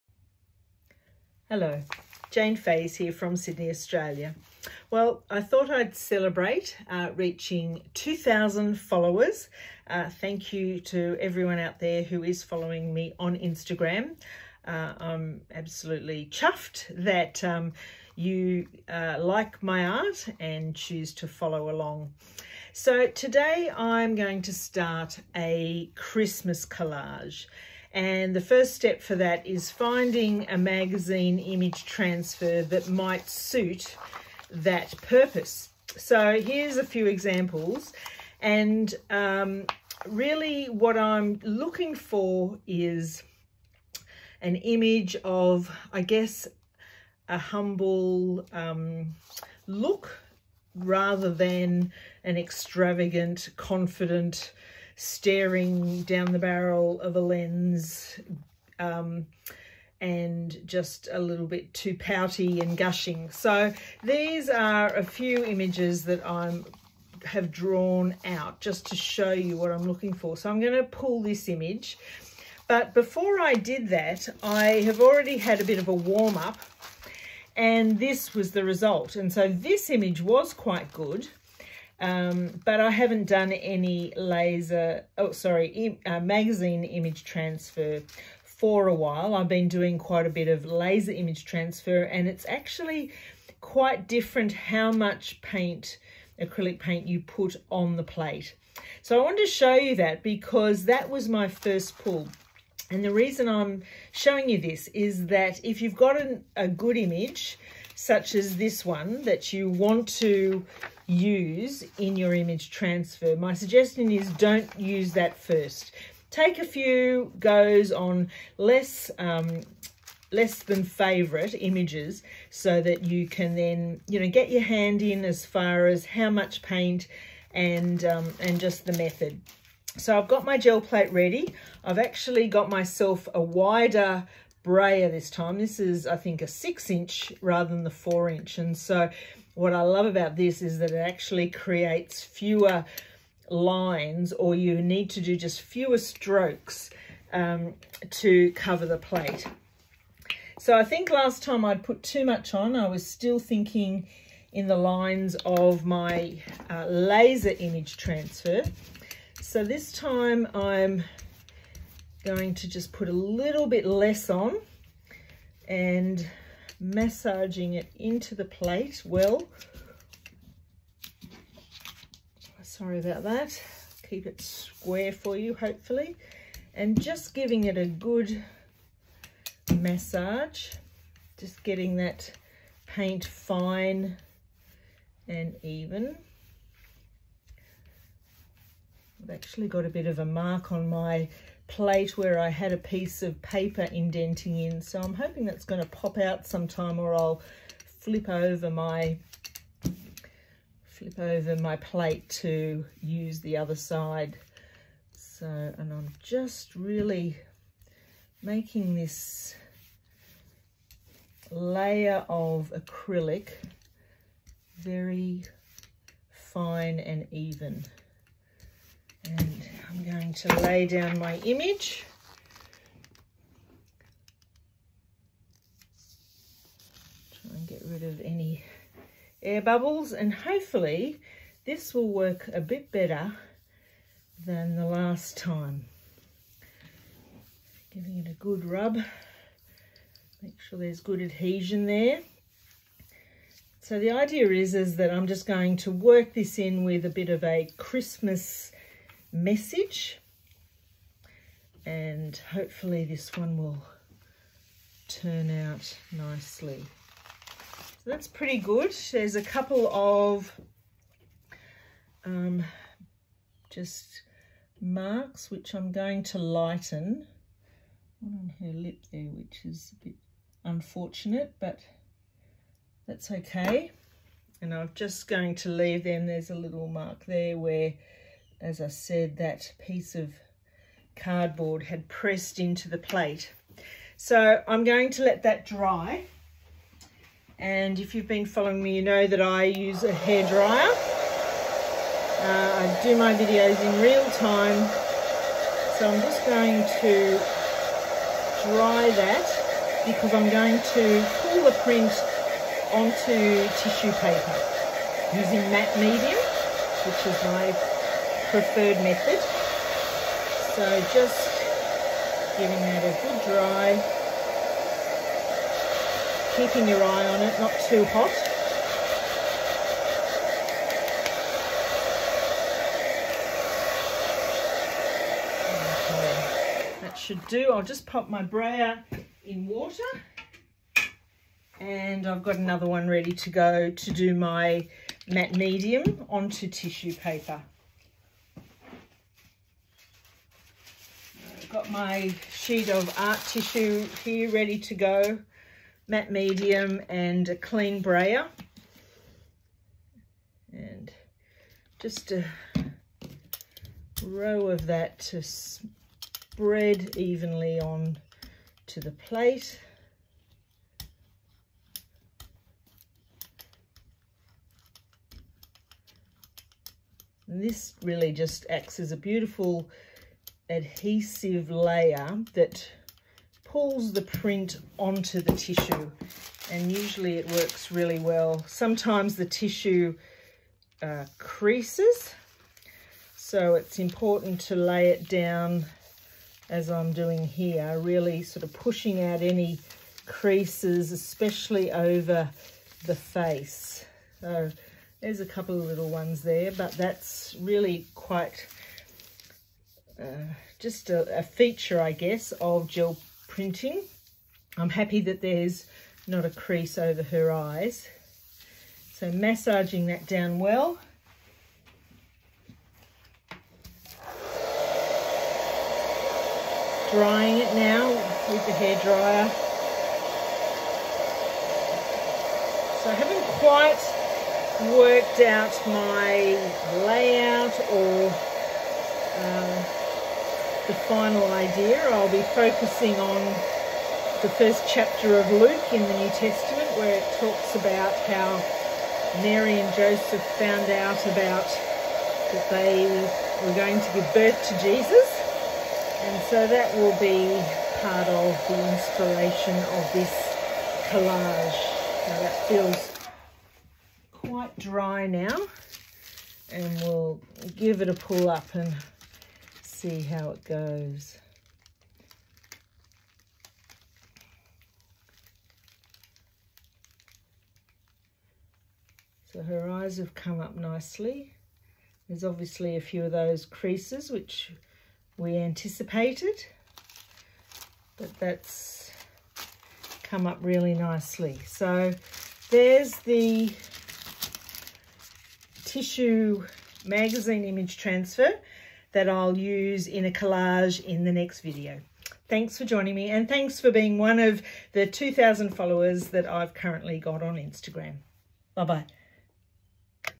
Creating a Christmas Collage - Video 1 of 3.
To begin my very non-traditional Christmas collage, I create a magazine image transfer by choosing an appropriate image then use the gellplate to make the transfer. This is the first of 3 videos.